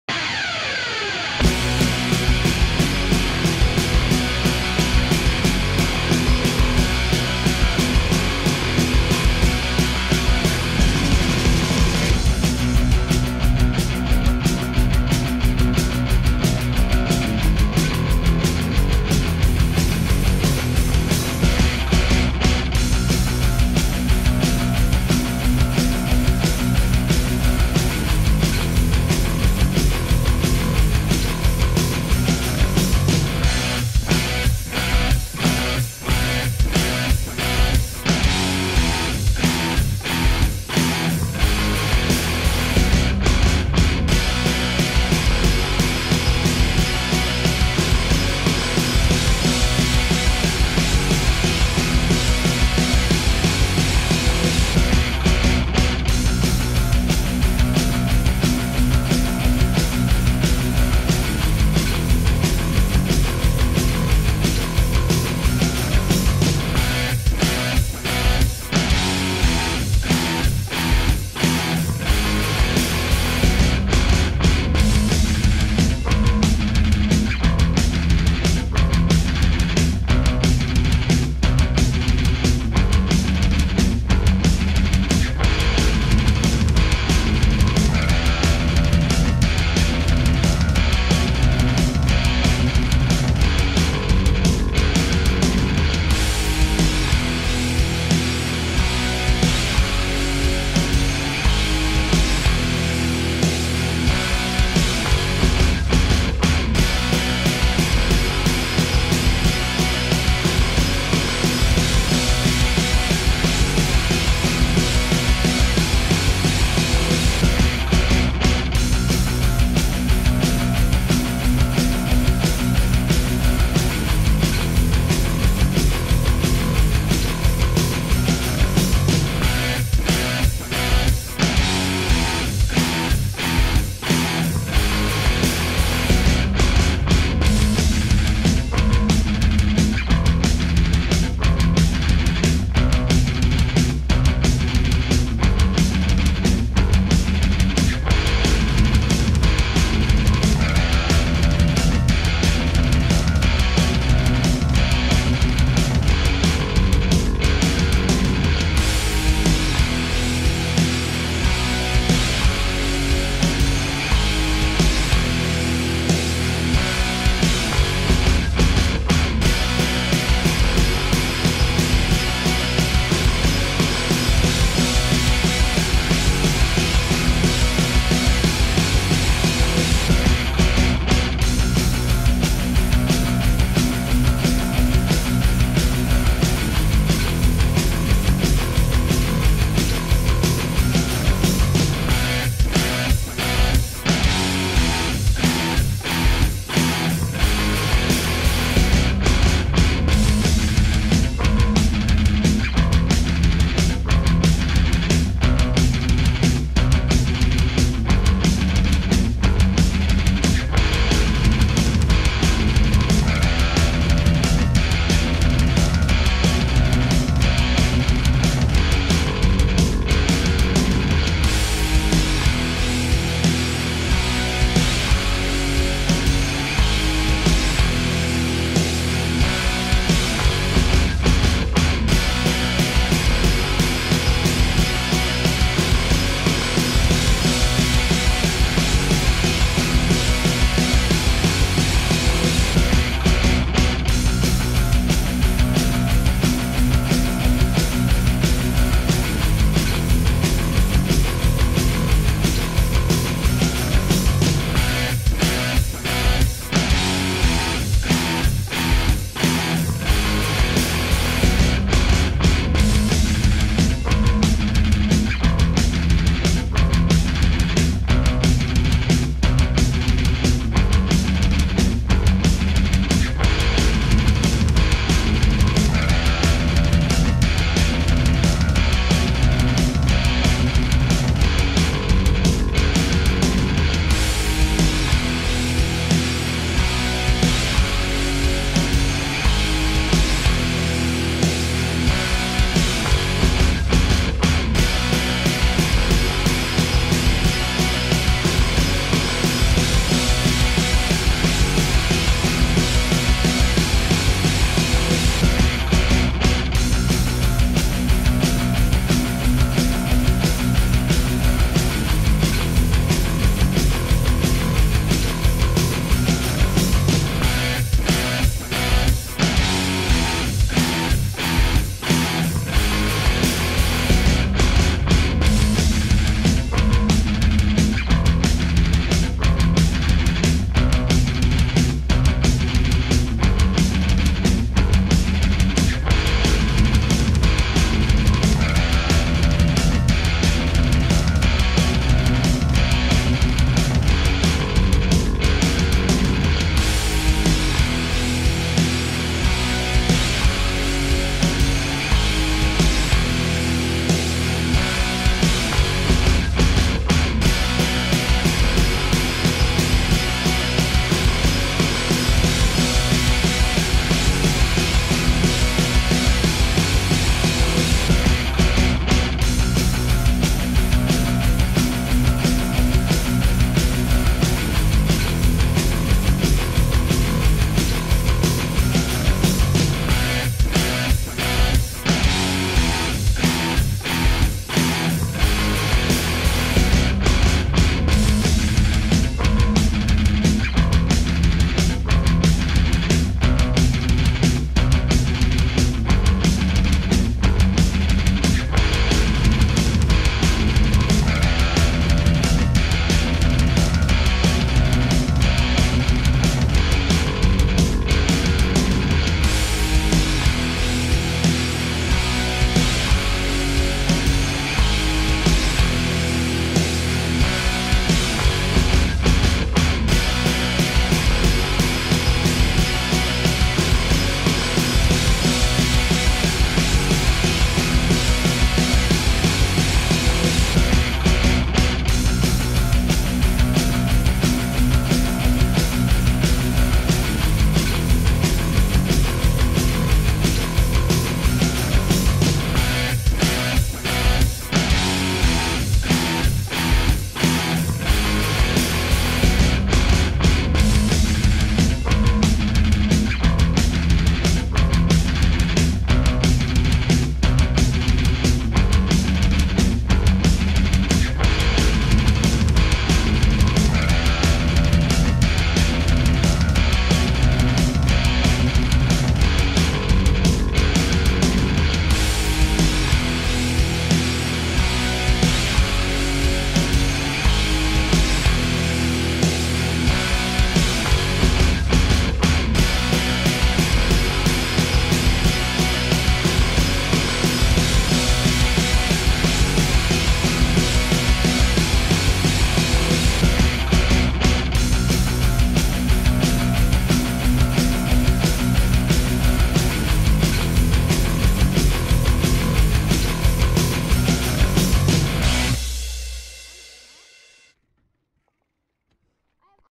red hill mtb trails crusher
sorry about all the starting and stopping and that i missed a part because i forgot to turn the gopro back on.